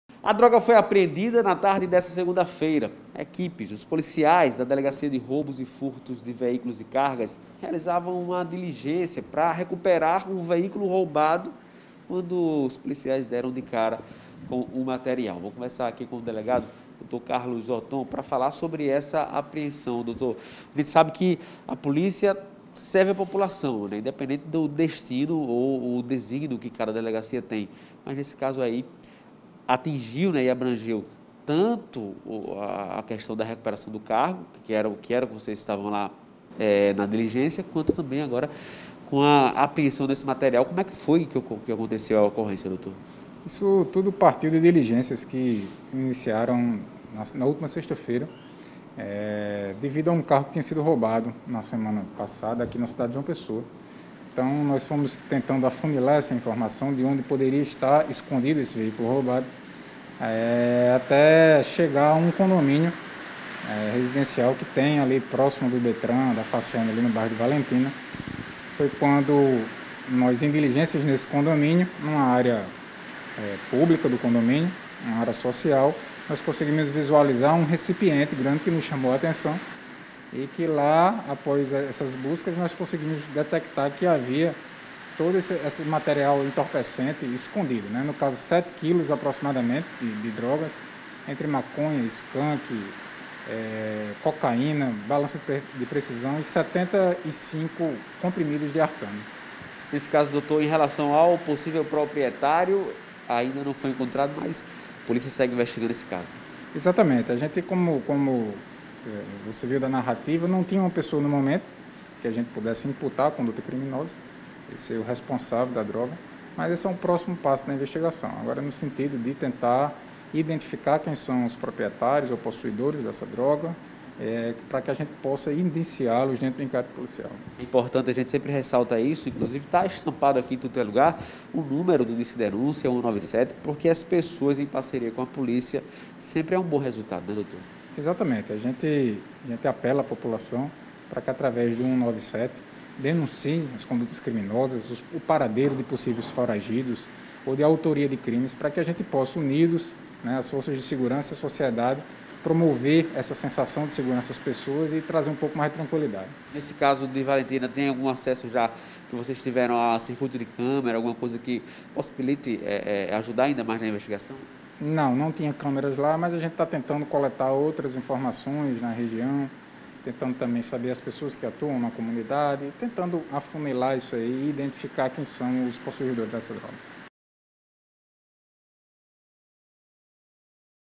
Apreensão de drogas no Valentina
A polícia conseguiu apreender drogas e um carro roubado no bairro do Valentina de Figueiredo. Betinho Nascimento traz as informações.